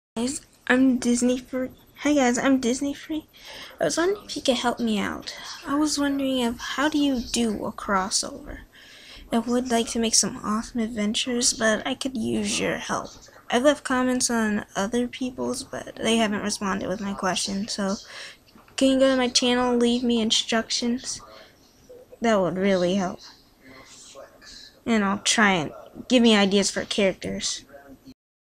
A Little Help Please!!!!!!!!
The video is unclear, but i could really use some on how to make crossover cause i want to do some awesome crossovers adventure like CityMaker and other,but need some instructions some could you leave intructions on my channel and idea for characters i could use. Please help me!:)